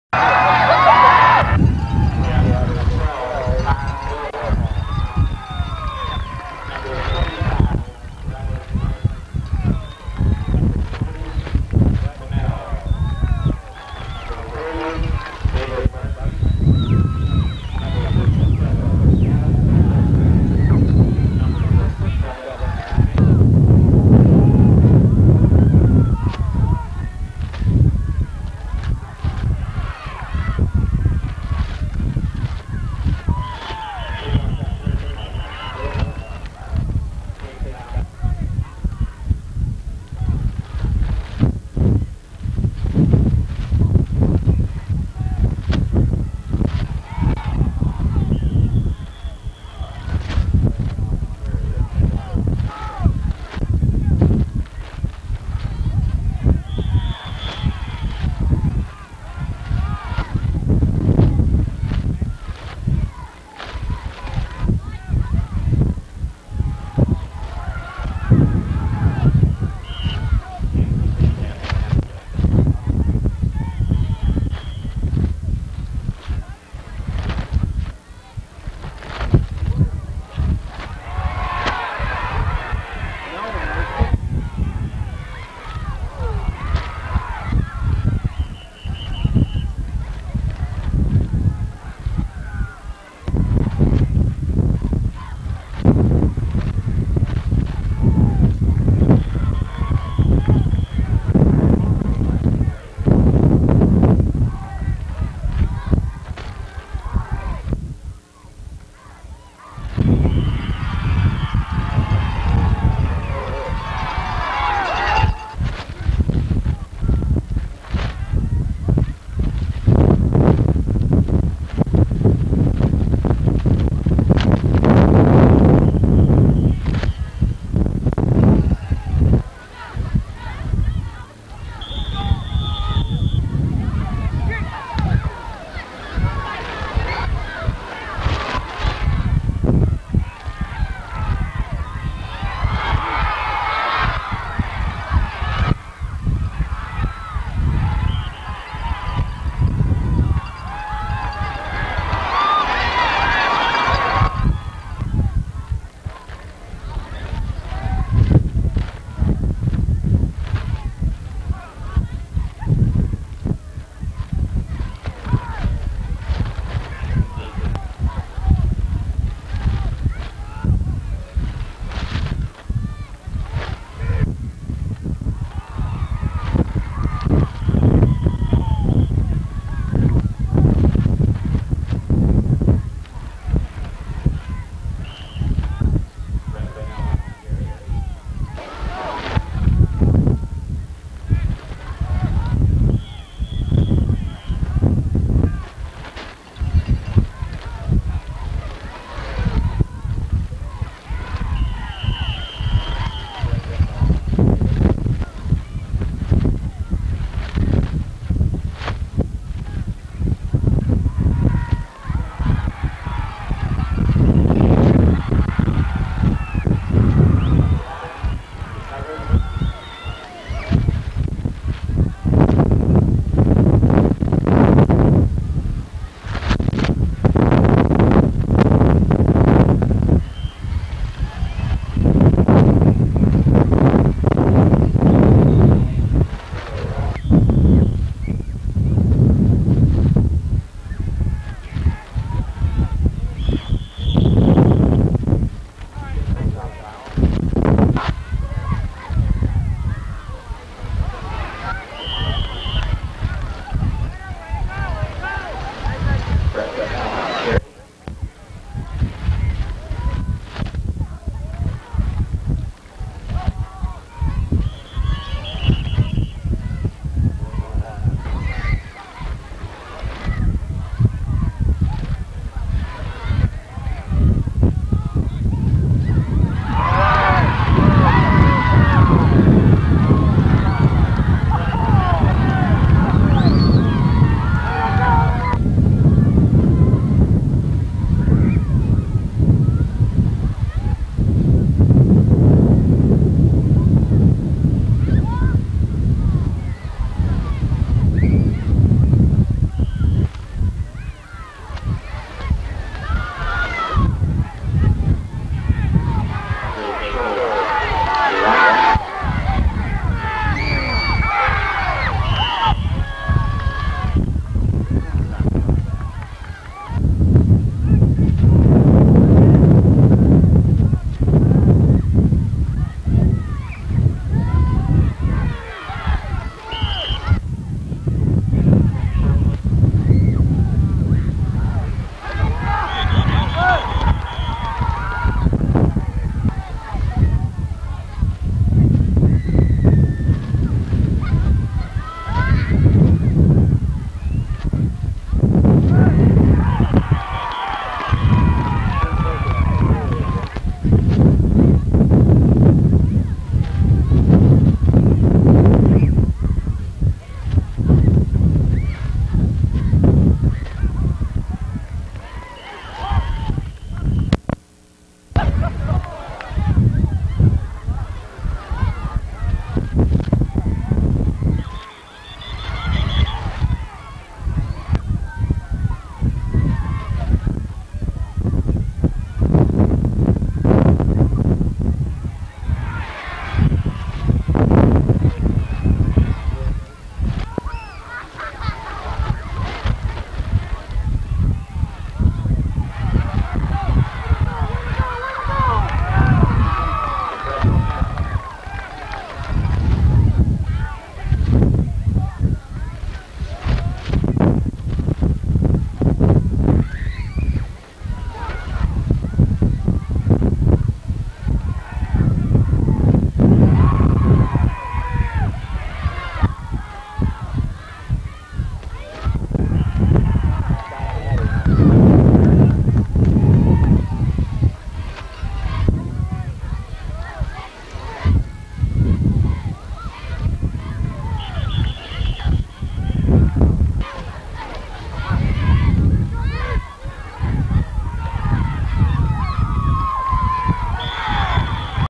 1988 WHS vs. LaMoille - 1st Quarter
Walnut vs. LaMoille in the ninth game of the 1988 regular season. Walnut defeated LaMoille 27-6.